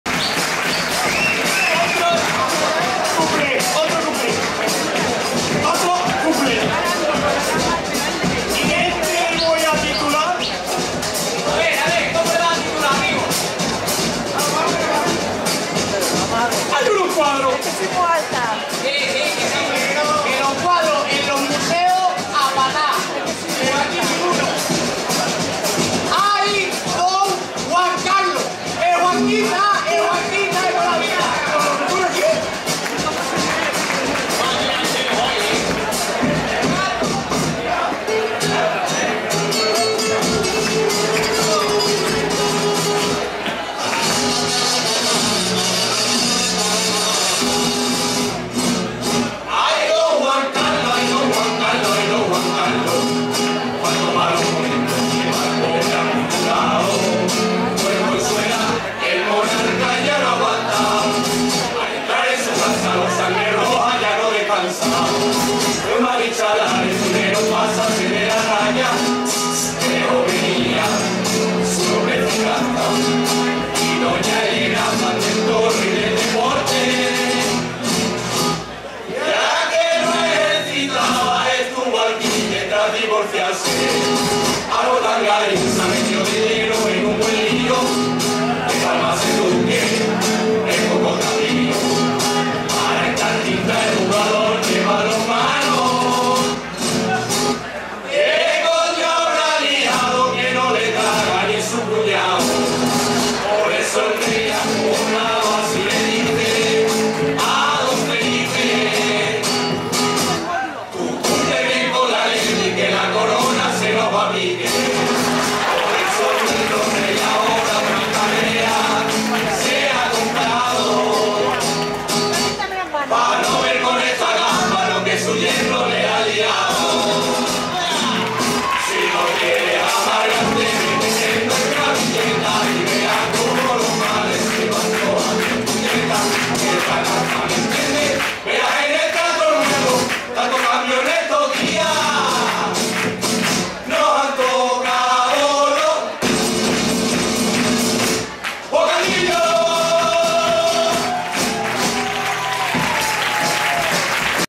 Los Ilustrados del Kiosco 05 Chirigota
-- Concurso de Murgas y Chirigotas en los Carnavales de Navalmoral de la Mata (Caceres)